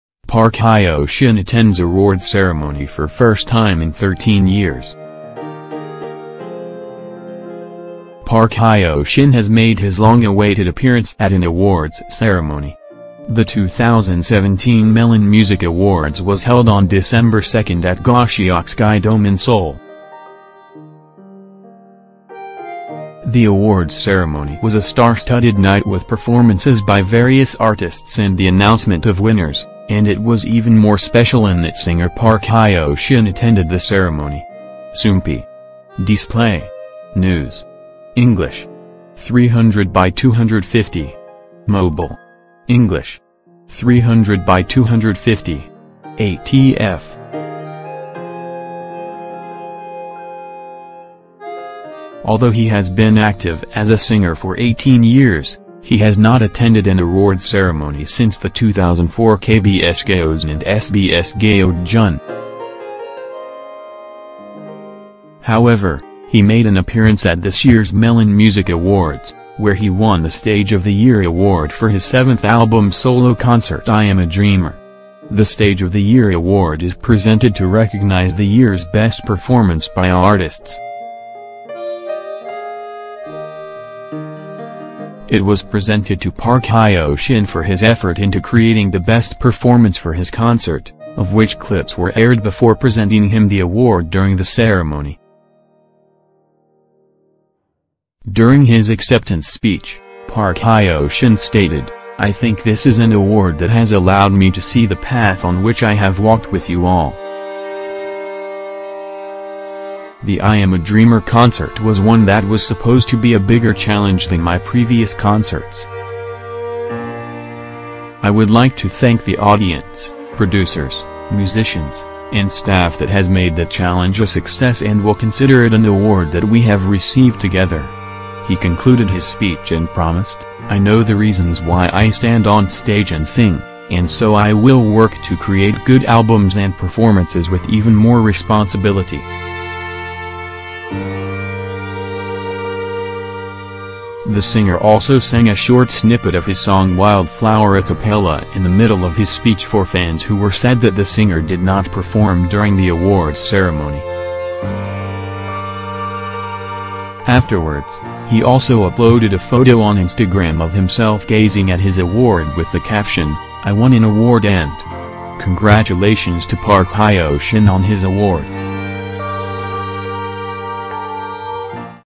Park Hyo Shin Attends Awards Ceremony For First Time In 13 Years(News)
Park Hyo Shin has made his long-awaited appearance at an awards ceremony!